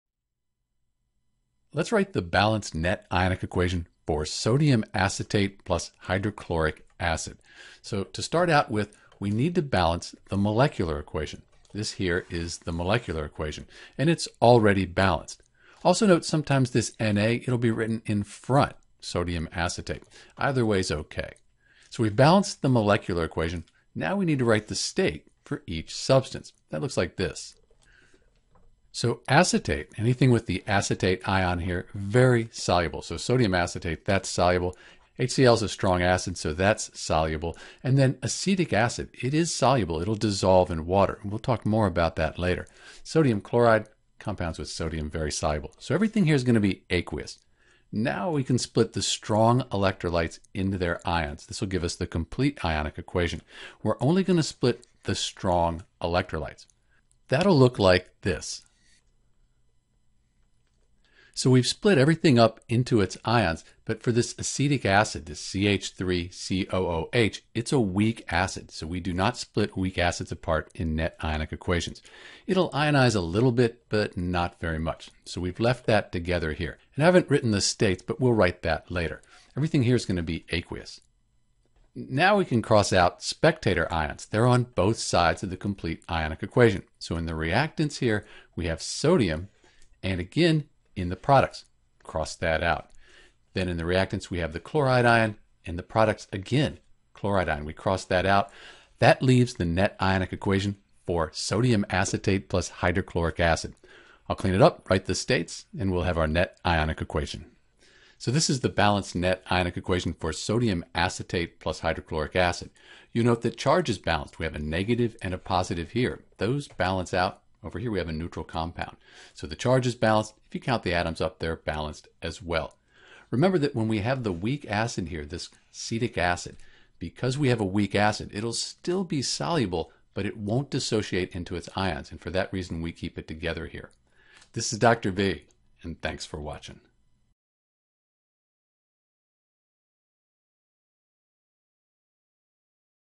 How to Write the Net Ionic Equation for CH3COONa + HCl = CH3COOH + NaCl
There are three main steps for writing the net ionic equation for CH3COONa + HCl = CH3COOH + NaCl (Sodium acetate + Hydrochloric acid). First, we balance the molecular equation. Second, we write the states and break the soluble ionic compounds into their ions (these are the strong electrolytes with an (aq) after them). Finally, we cross out any spectator ions. These are the ions that appear on both sides of the ionic equation.

Important Skills

More Practice

General Steps:

1. Write the balanced molecular equation.
2. Write the state (s, l, g, aq) for each substance.
3. Split soluble compounds into ions (the complete ionic equation).
4. Cross out the spectator ions on both sides of complete ionic equation.
5. Write the remaining substances as the net ionic equation.